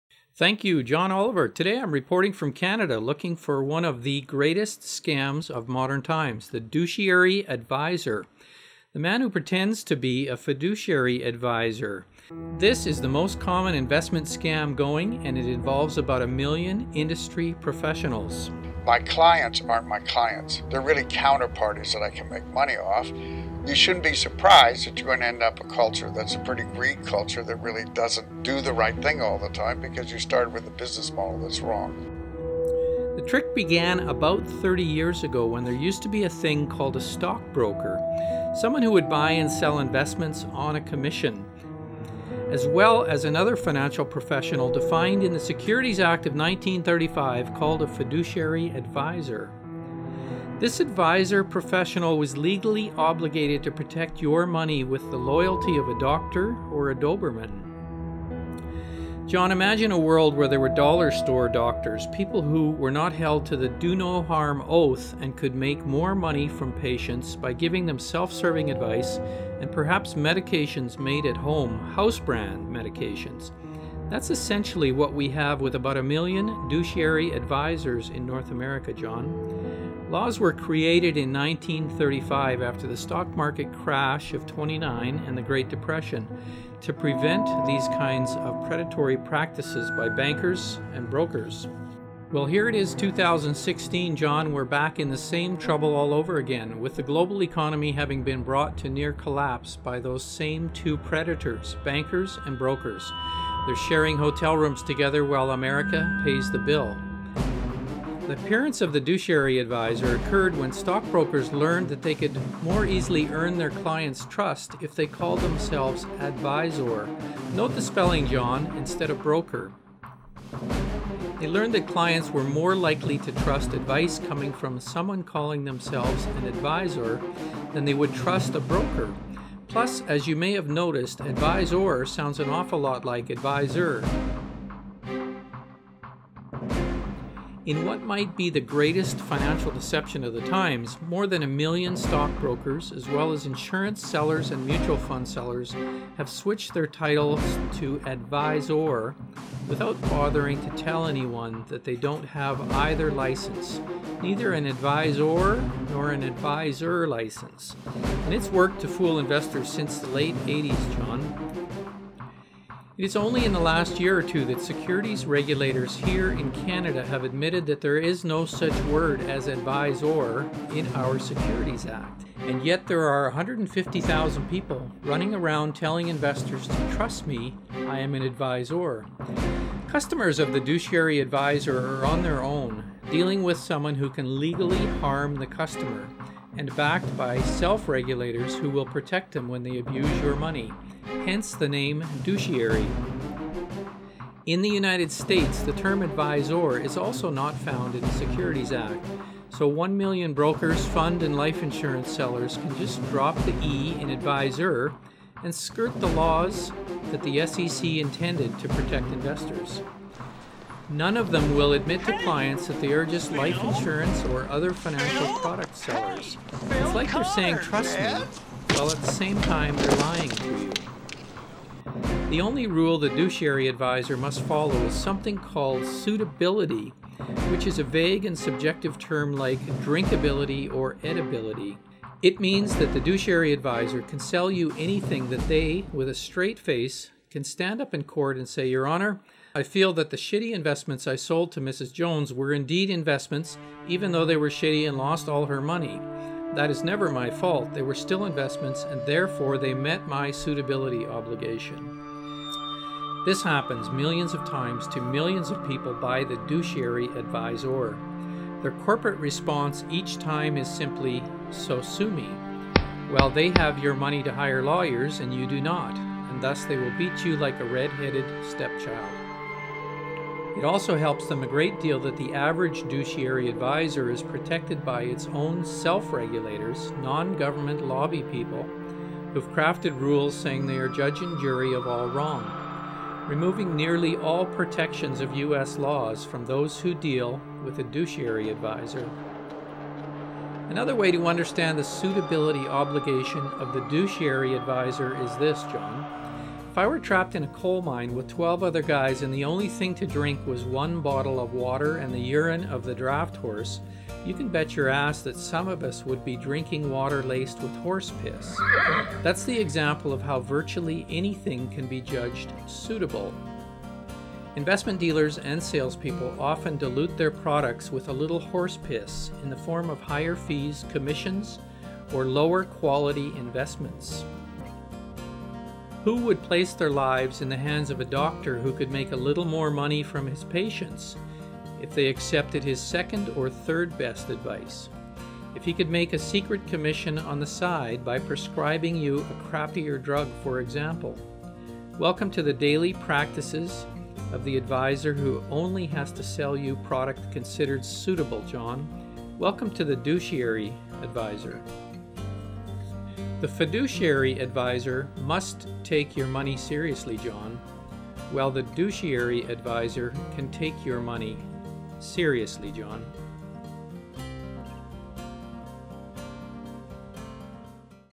The "Douche-iary" Investment Advisor
A short 6 minute clip intended as a John Oliver story pitch. Created by a  recovering Canadian broker who hopes to warn the public about the greatest financial predators on our planet. Their own investment "advisor".

Namely banks, investment dealers, and their millions of lawyers and regulatory handmaidens.  The harm done by these groups, acting in tandem, in a systemic manner, is doing annually more harm to North American than the financial harm done by ALL OTHER CRIMES OF ALL KINDS, BY ALL CRIMINALS.

Currently they are getting away virtually free of application of any criminal codes or systems of justice.  They are, quite simply looting the land, while enjoying power and status which places them above the law.  Find me @RecoveredBroker  My permission is given to share, edit, improve or broadcast this video in any way that may increase public awareness and improve the protection of investors.